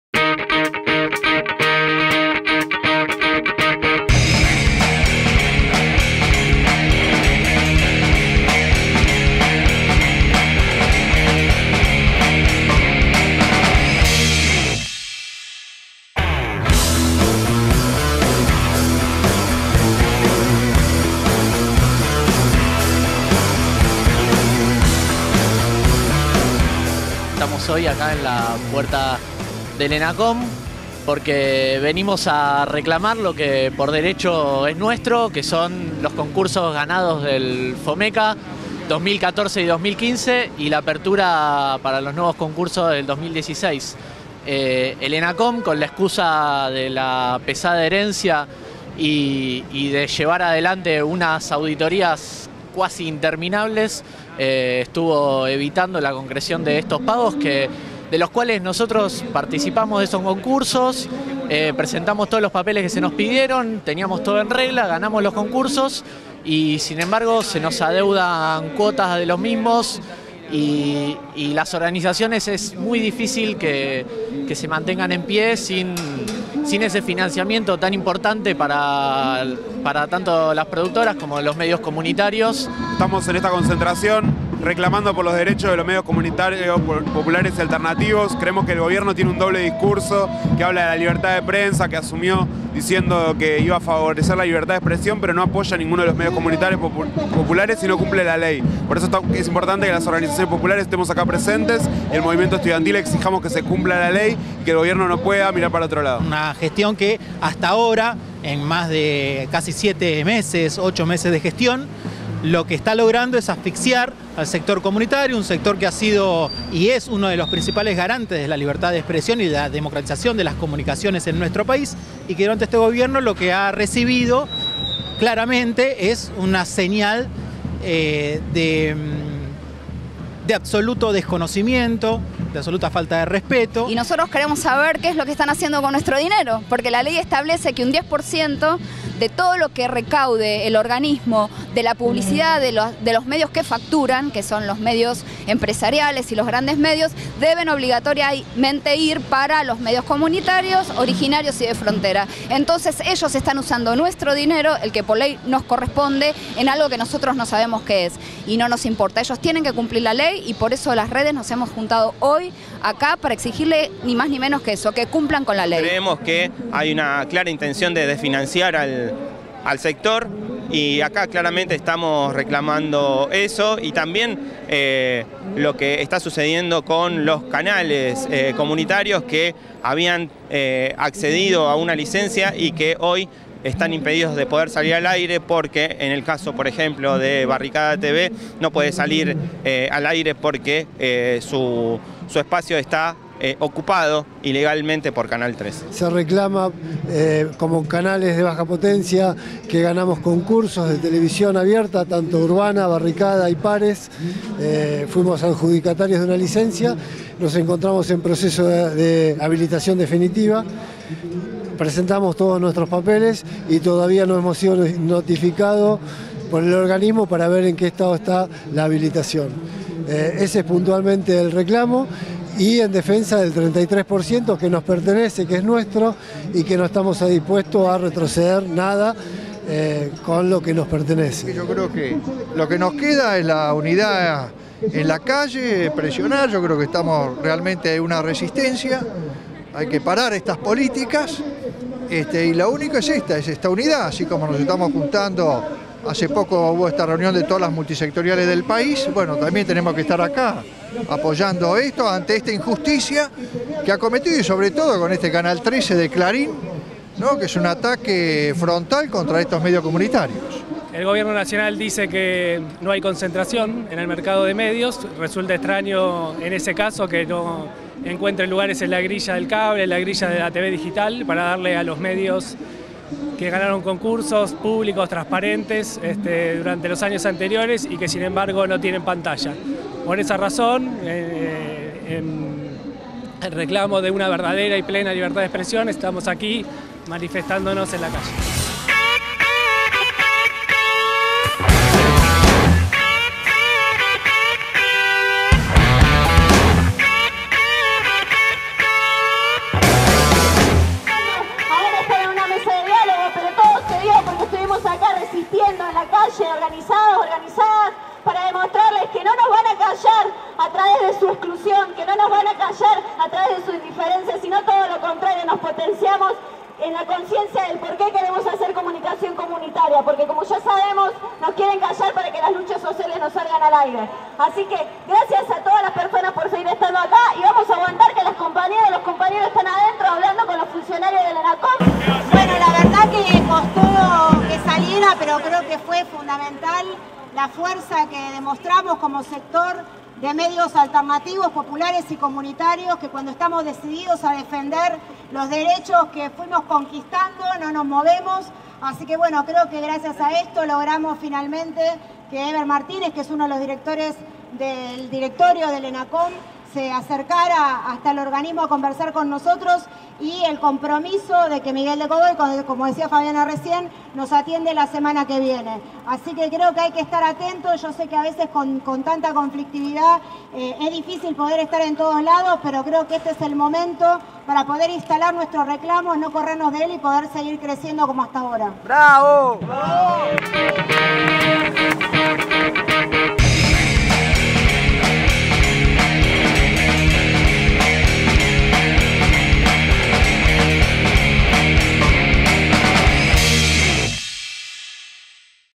Medios populares frente al ENACOM: "el 33% nos pertenece y no estamos dispuestos a retroceder"
El día 27/07 distintos espacios de medios alternativos y populares nucleados en Interredes, se movilizaron a las puertas del Ente Nacional de Comunicaciones.
Acompañados de una gran cantidad de organizaciones sociales y políticas, exigieron al organismo que se paguen las cuotas FOMECA adeudadas por los concursos ganados en el año 2015., que se cumpla con la Ley 26.522 y se ejecute el presupuesto destinado a nuestro sector del año 2016, que se habilite a los canales comunitarios que ganaron concursos públicos en televisión digital, que se cese la persecución legal y los decomisos a los medios comunitarios, que se respete el 33 por ciento del espectro destinado a los medios sin fines de lucro y se participe a todo el sector en la discusión de una nueva ley de medios.

Debido a la gran presión que se ejerció,  representantes del sector comunitario lograron fueron atendidos por Heber Martínez, miembro del directorio, quien comprometió a Miguel de Godoy a realizar una reunión el día jueves 4/08.